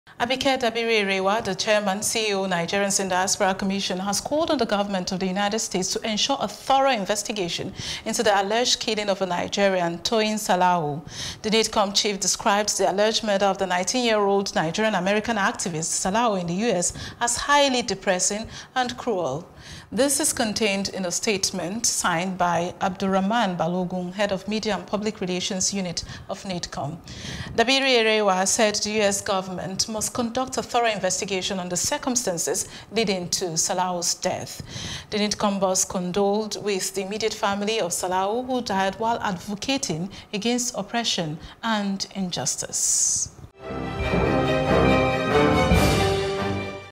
Dabiri Erewa Tasks U.S. Govt. On Killing Of Nigerian-American Salau
Abike Dabiri-Erewa, the Chairman/CEO, Nigerians in Diaspora Commission, has called on the government of the United States to ensure a thorough investigation in the alleged killing of a Nigerian, Toyin Salau.

The NiDCOM chief described the alleged murder of the 19-year-old Nigerian-American Activist, Salau in the U.S as highly depressing and cruel.